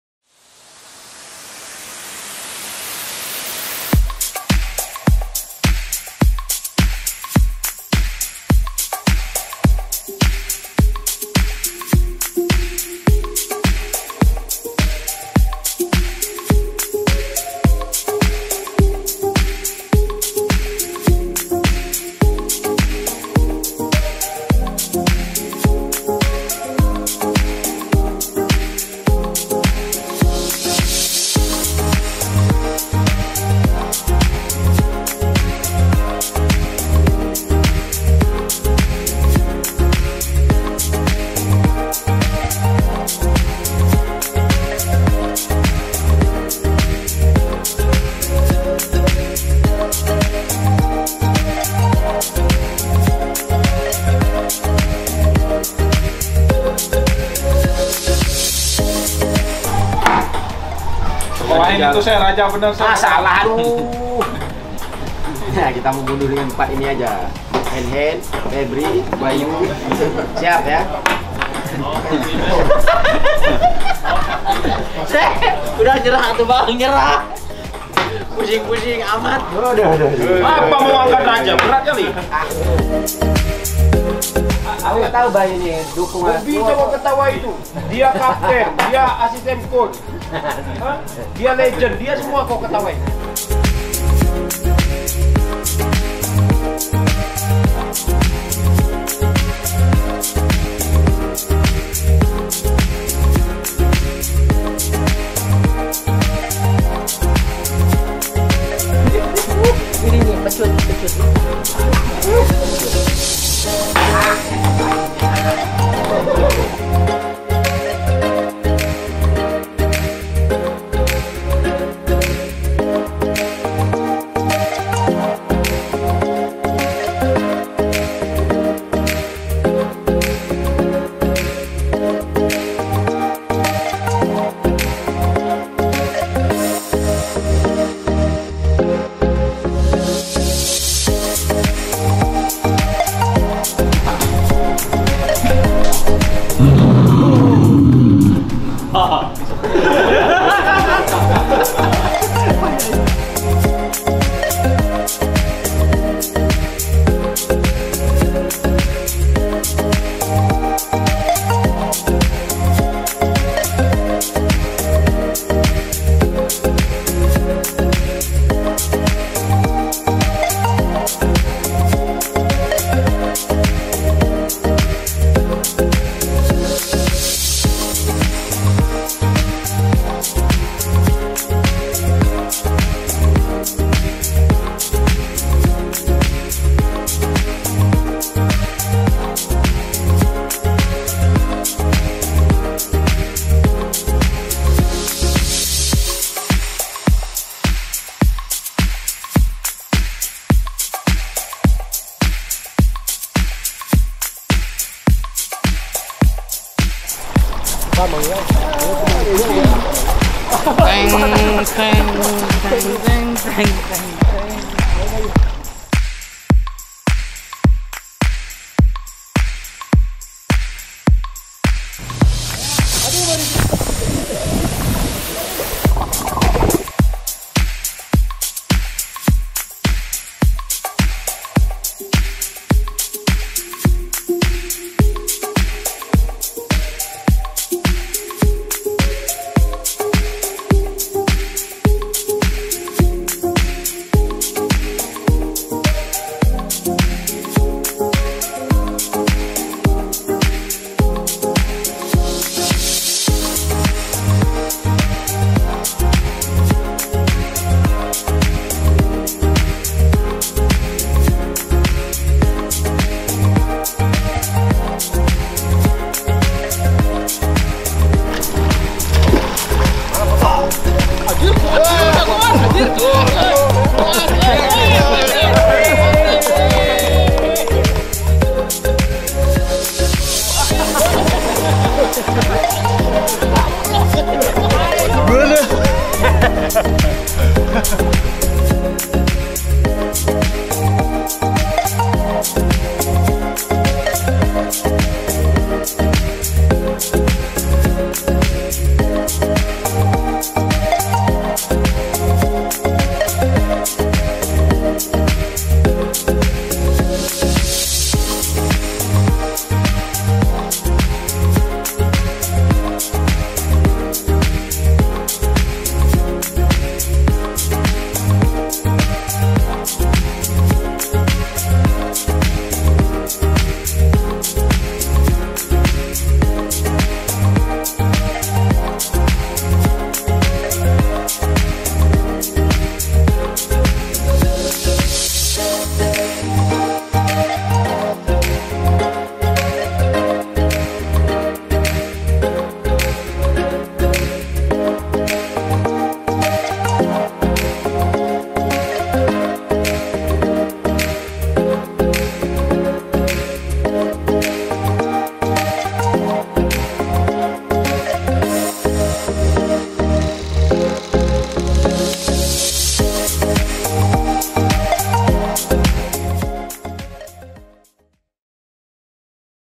Recovery Session|Nikmati Lolos Semifinal
Setelah memenangi laga melawan Persebaya, PERSIB kembali memulihkan energinya di kolam renang dan menikmatinya sambil menanti calon lawan di Semifinal Piala Menpora 2021.

Ini adalah kanal YouTube resmi dari PERSIB, klub terbesar di Indonesia dan Asia yang berasal dari Bandung.